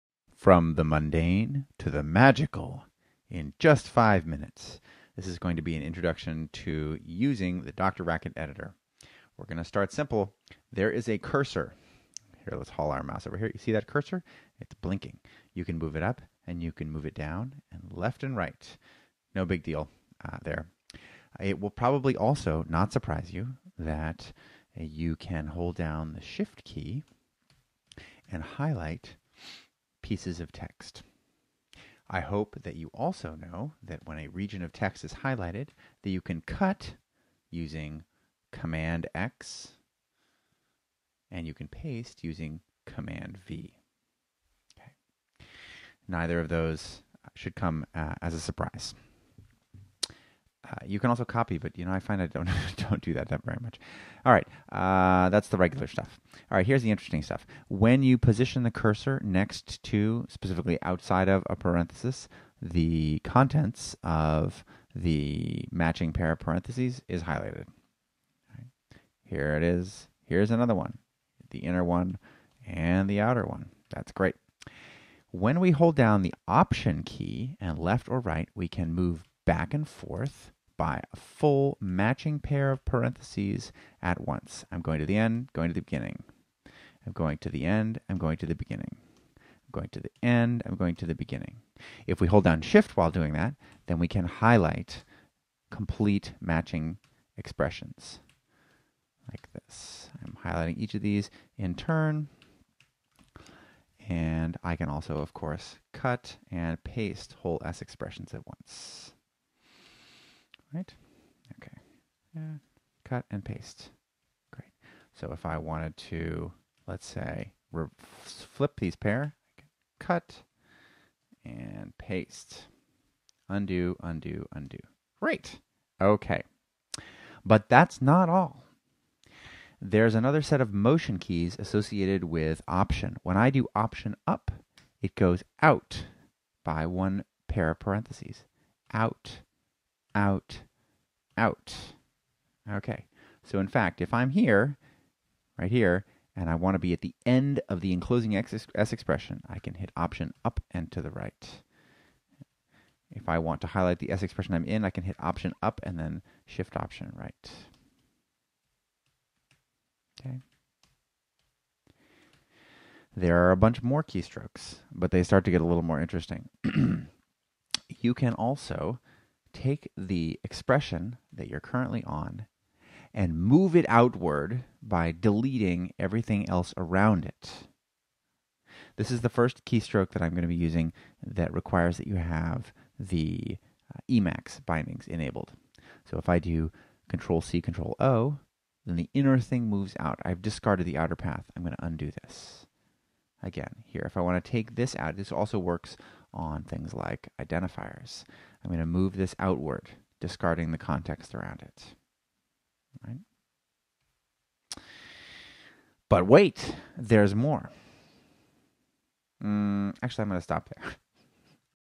DrRacket 01.5 -- Keyboard Editing 1
Starting with arrow keys, moving upward through s-expression based movement & cutting & pasting, ending with c-x;c-o